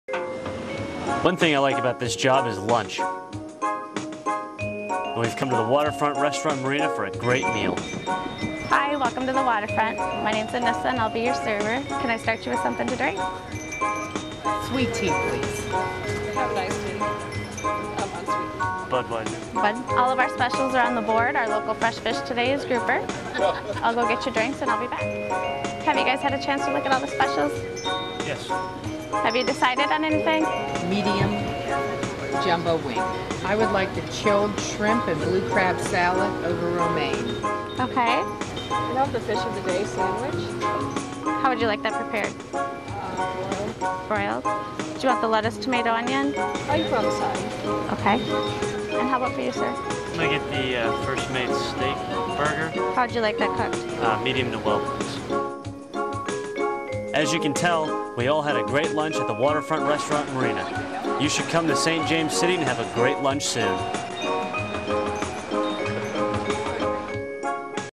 Boater's Treasures TV Lunch at the Waterfront Restaurant. Fort Myers Beach Video
Chris and his crew at Boater's Treasures TV crew have lunch at the Waterfront Restaurant in St. James City, Pine Island Florida. Fort Myers Beach Video